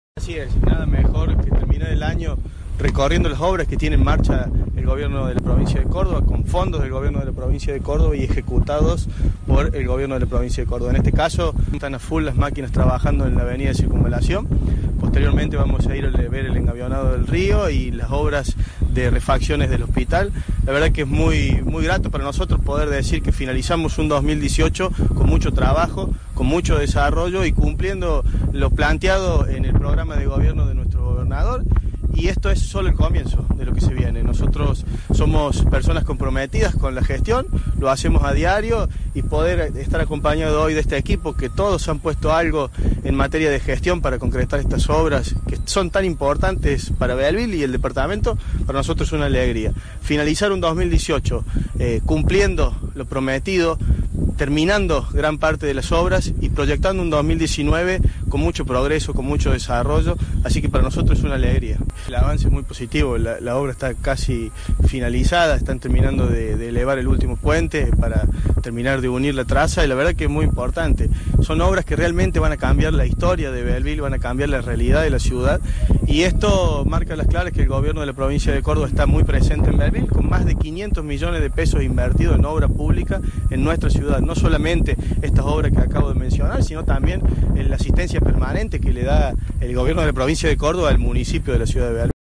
Recorrida por obras de la provincia en Bell Ville
El subsecretario de Cooperativas y Mutuales del Gobierno de Córdoba, Abraham Galo, acompañado por dirigentes locales, visitaron importantes obras que la provincia ejecuta en Bell Ville.